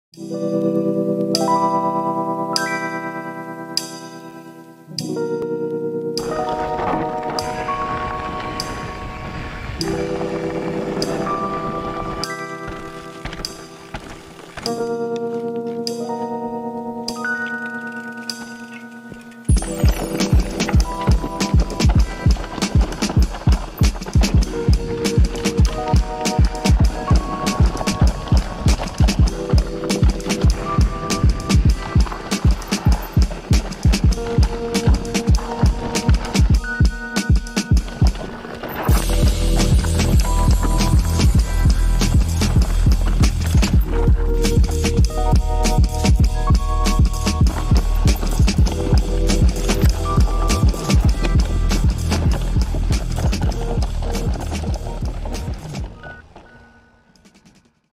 Marina di Camerota Sentiero di S. Iconio
Primo piccolo giro a Marina di Camerota.
Un sentiero davvero molto bello e a tratti tosto. Strappetti importanti in salita e gradoni affilati in discesa.